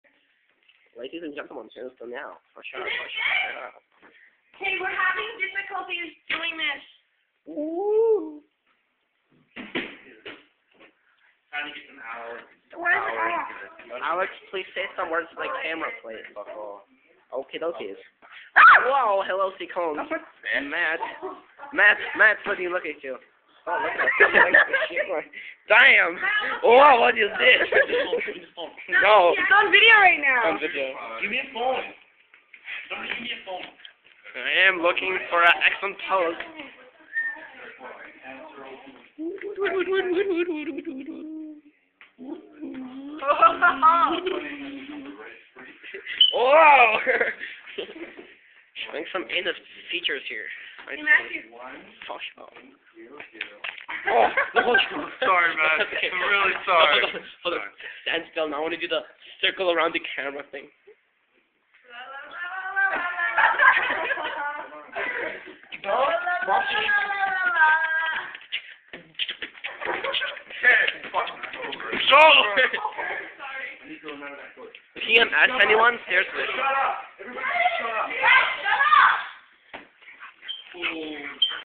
photo copying fun
at a cadets meeting on a wednesday trying to help photo copy but the machine dies on us but we still had fun lol